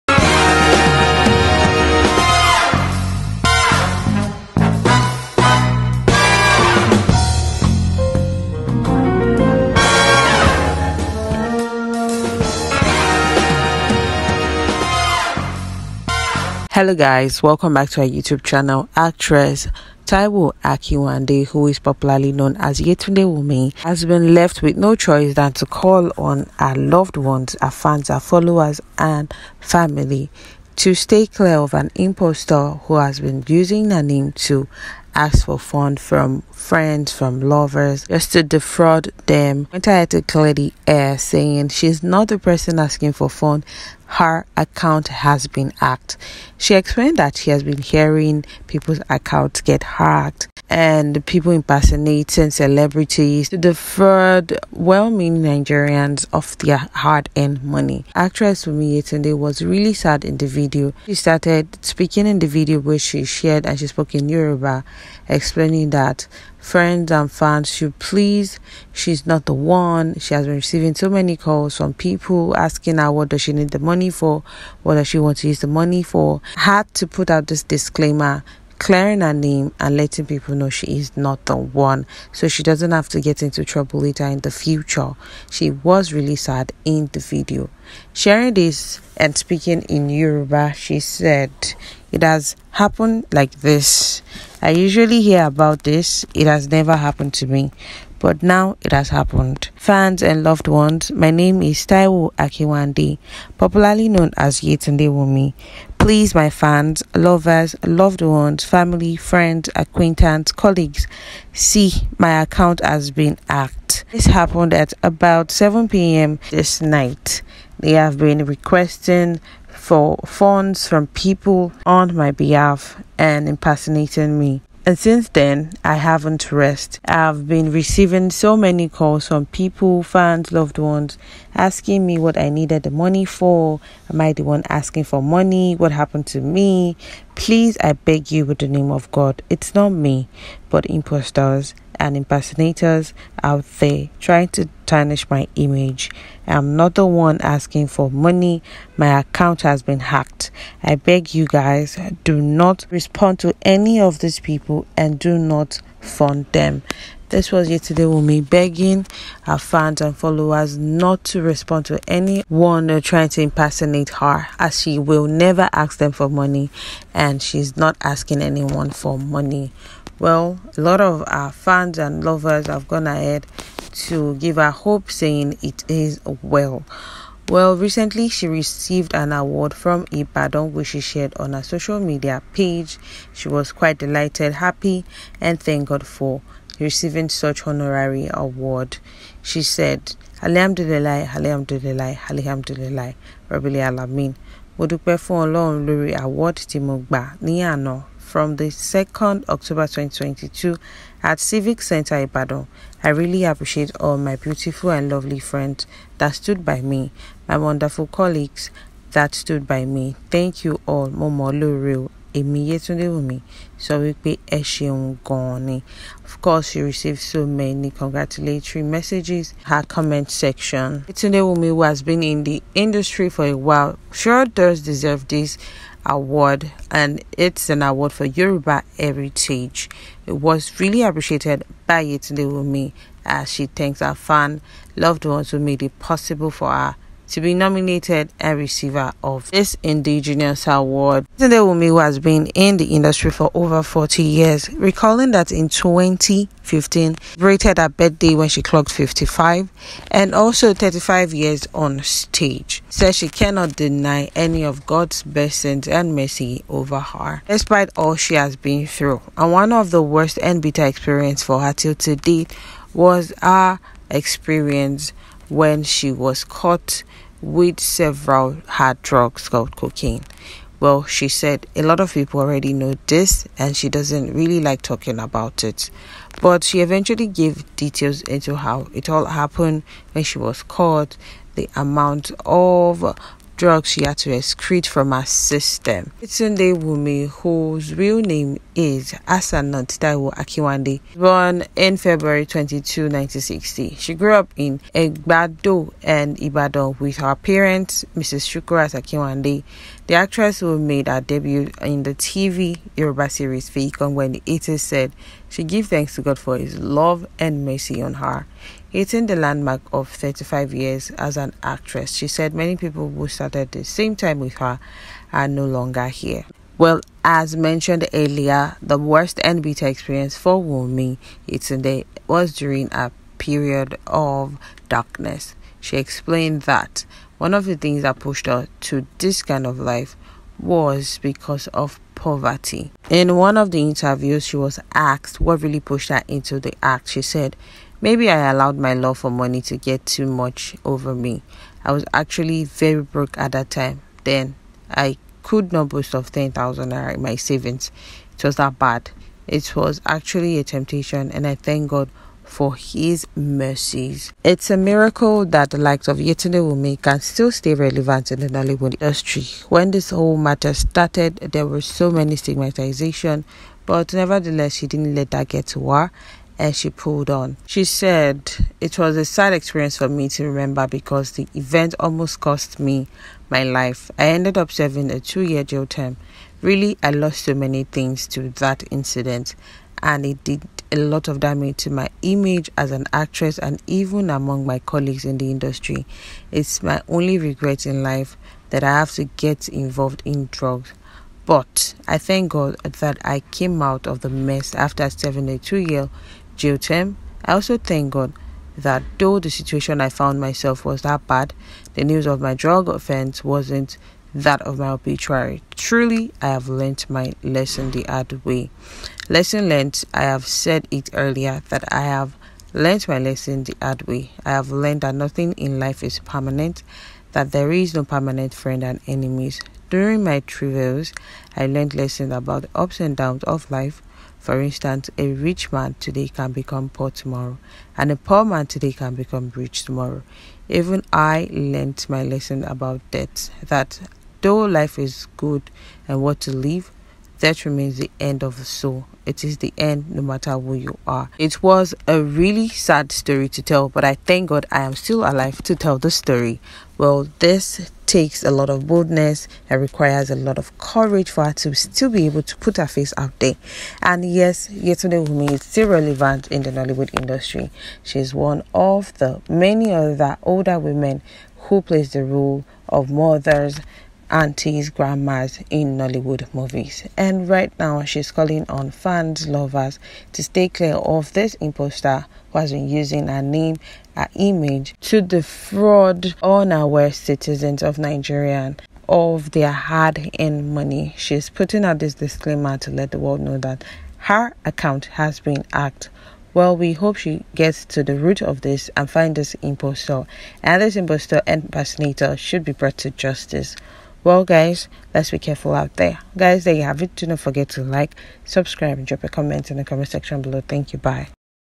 ‘Please Help Me O’ Actress Yetunde Wunmi Cries Out, Begs Fans & Colleagues To ..
Veteran Yoruba Nollywood actress, Taiwo Akinwande, popularly called Yetunde Wunmi, has lamented about the unpleasant issue she had with fräudsters.